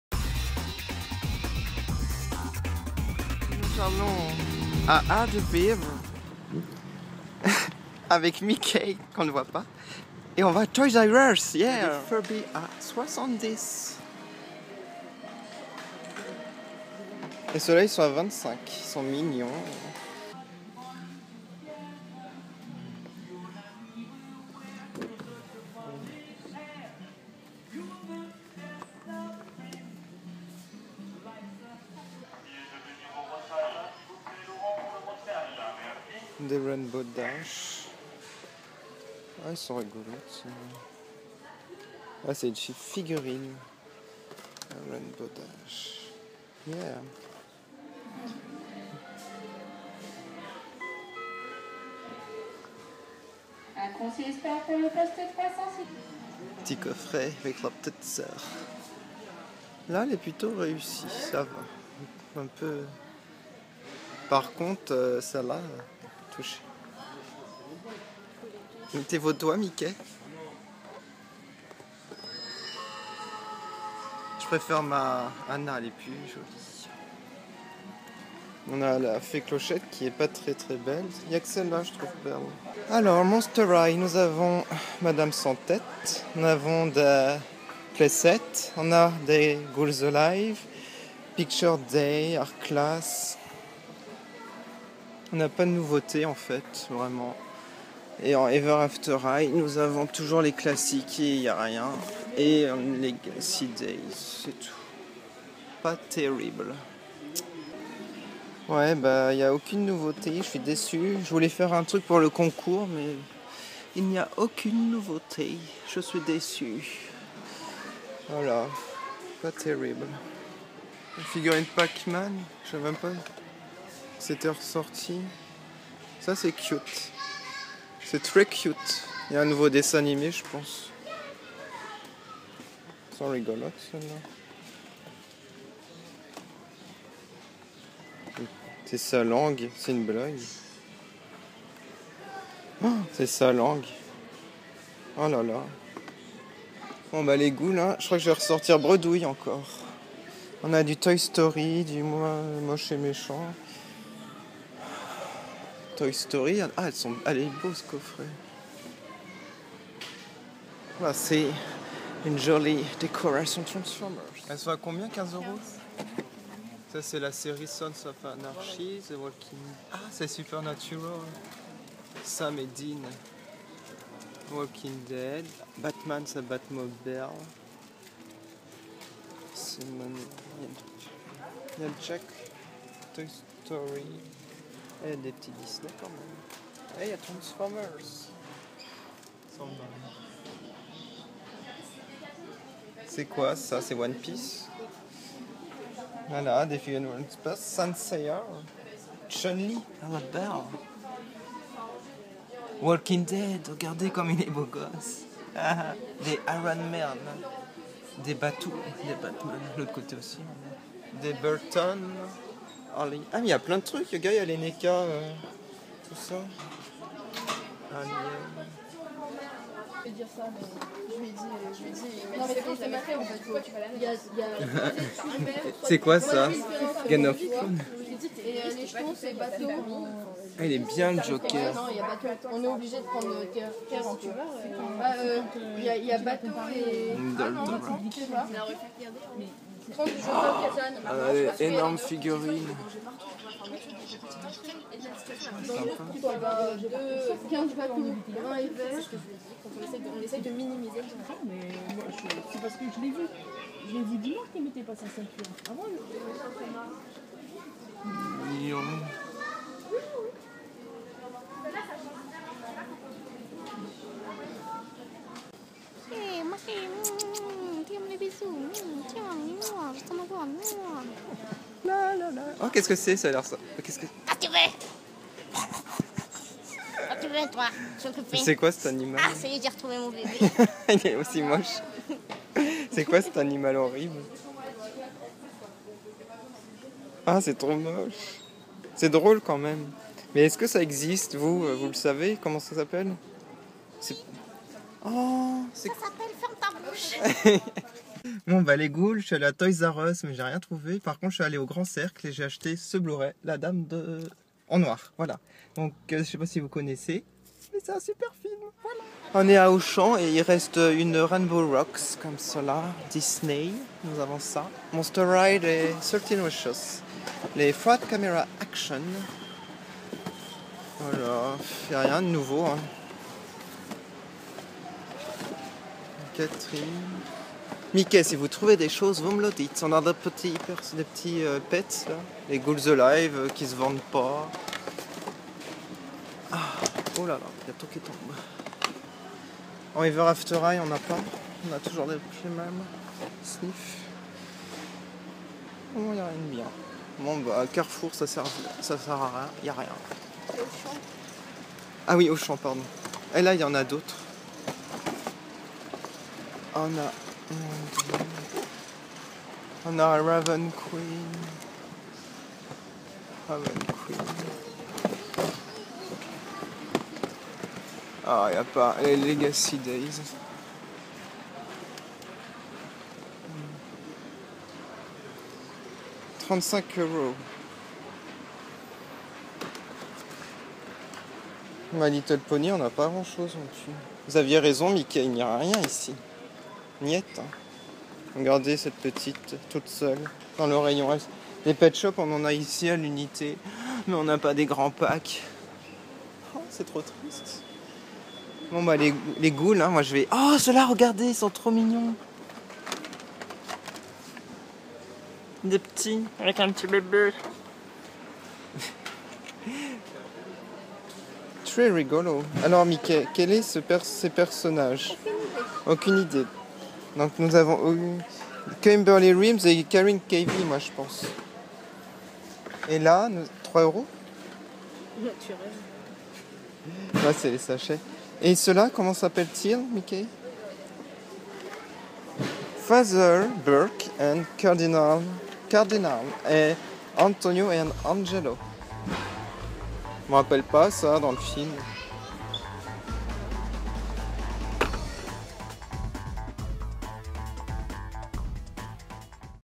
Erikku Dolls Hunting 28 - Toys'R'Us - Le Grand Cercle
Aujourd'hui une Dolls Hunting à Toys'r'us et Le Grand Cercle à Art de Vivre à Eragny puis à Auchan à Taverny!!